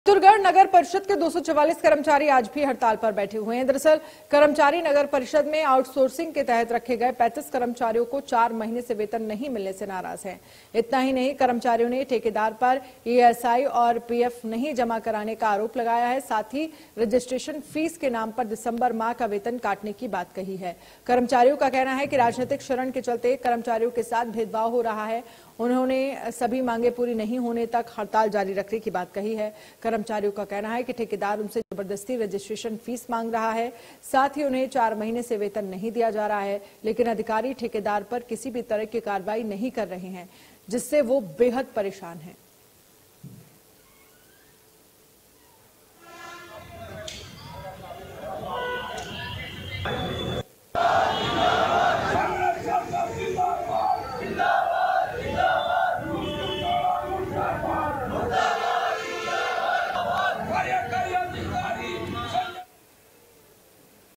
Bahadurgarh News : आउटसोर्सिंग के 35 कर्मचारियों को नहीं मिला वेतन || India News Haryana
| For more breaking news keep watching India News Haryana |